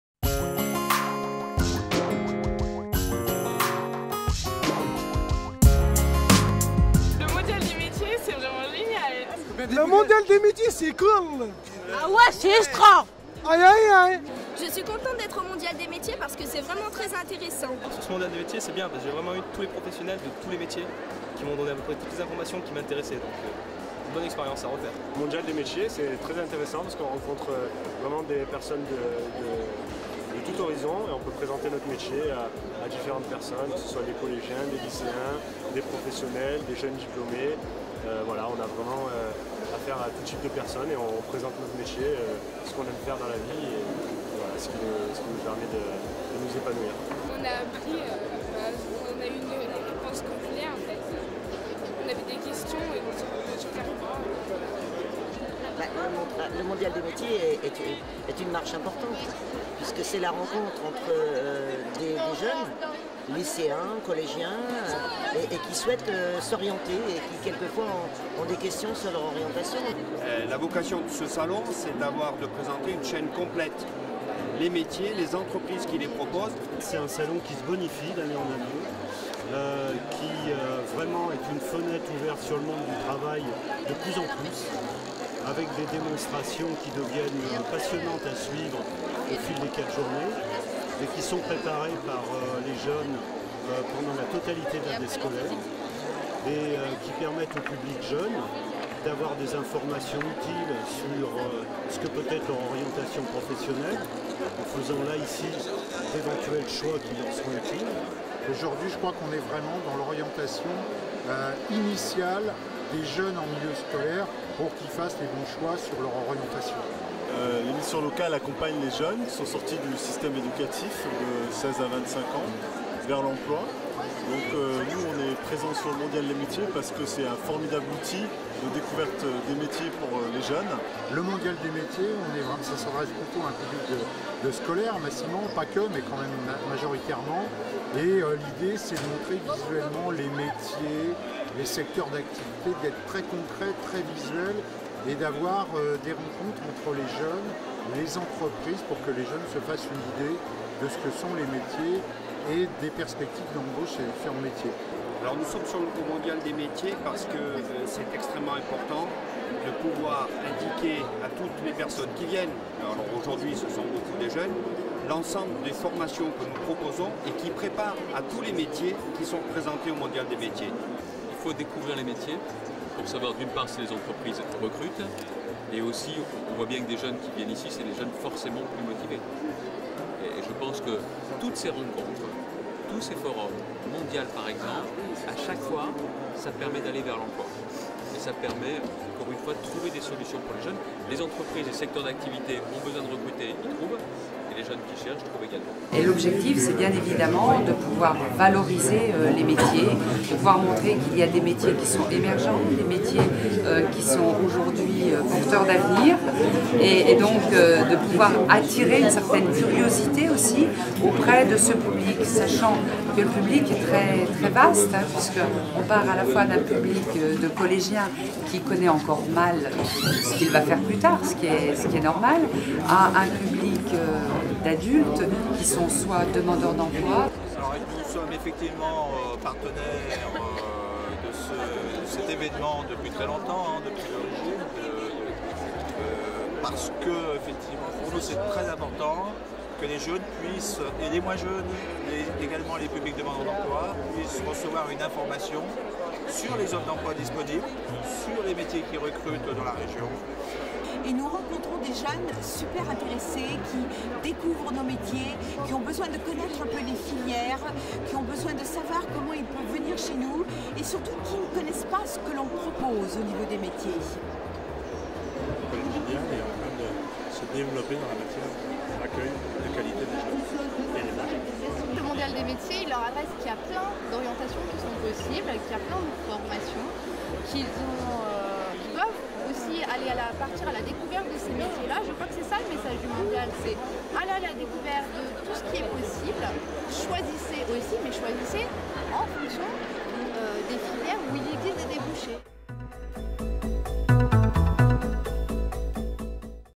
Inauguration du Mondial des Métiers 2012
Les partenaires institutionnels, l'Etat et la Région s'illustrent dans l'inauguration du Mondial des Métiers.